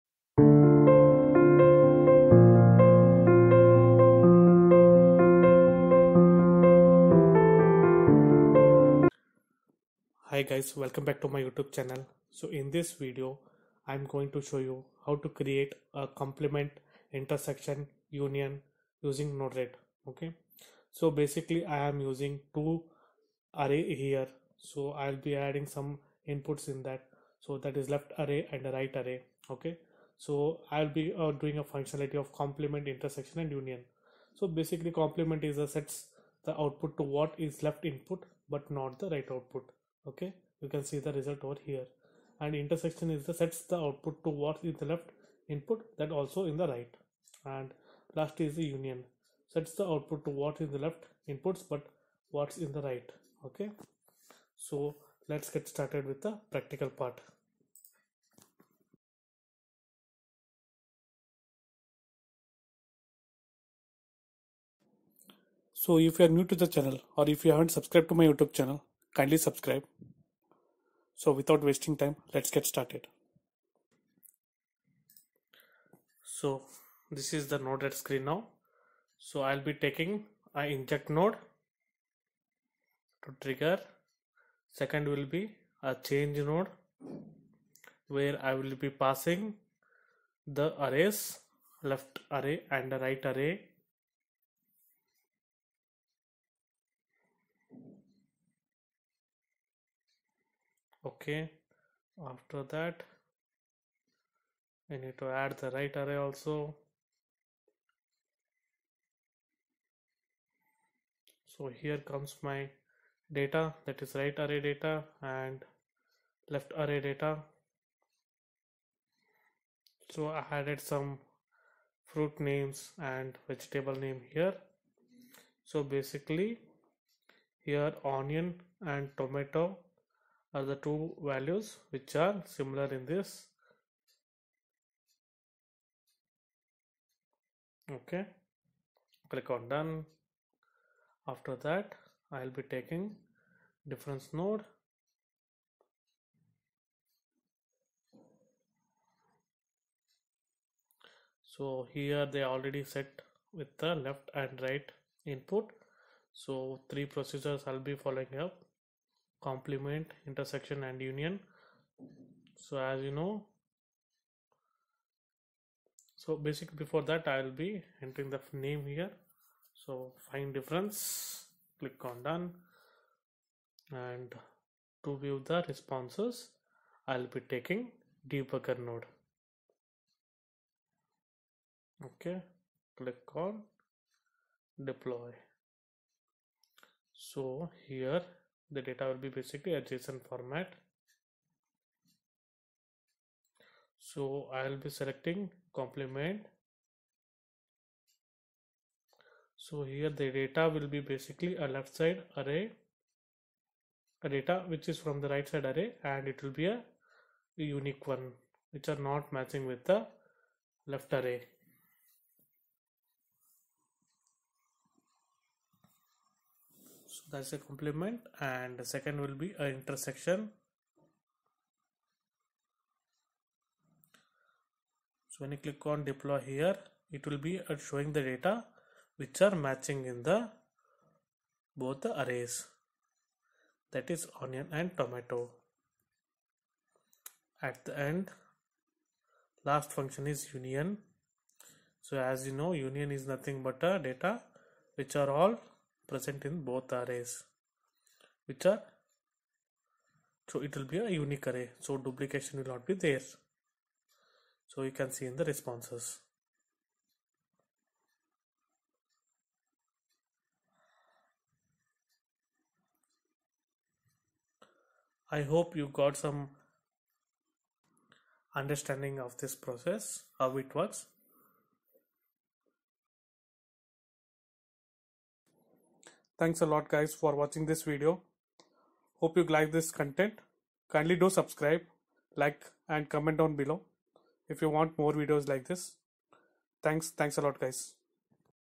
Learn Array Functions, Methods, and NodeRed in 3 Easy Steps
Hi Folks

in this video i am showing you how you can work on JavaScript array using NODERED with faster and simple way without coding

JavaScript Array Functions
JavaScript Array Methods
All about JavaScript Array